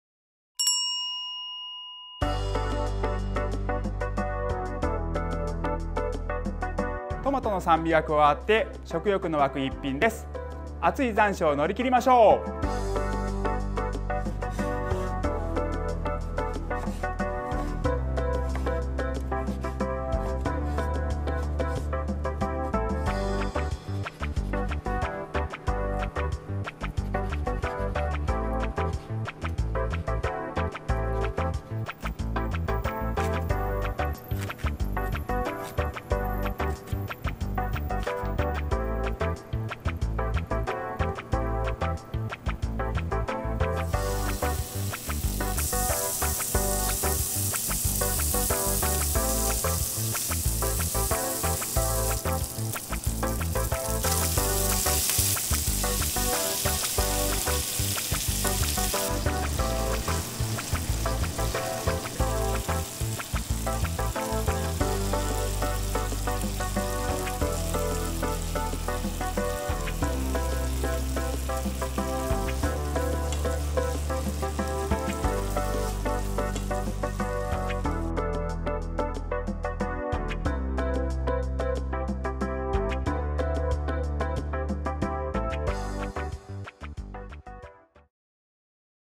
トマトと豚肉のキムチ炒め［1人分250kcal］［中浜良太郎先生］
トマトは火を通しすぎないように♪

トマト　　　　　…150g
豚ばら薄切り肉　…80g
白菜キムチ　　　…80g

Ａ
きび砂糖　　　　…小さじ1/2

◇作り方
1) トマトはひと口大に切り、豚肉は3cm幅に切り、白菜キムチは1cm幅に切る。
2) フライパンにごま油を熱し、豚肉を入れて炒め、色が変わったらトマト・キムチ・(Ａ)を入れて中火で1～2分炒める。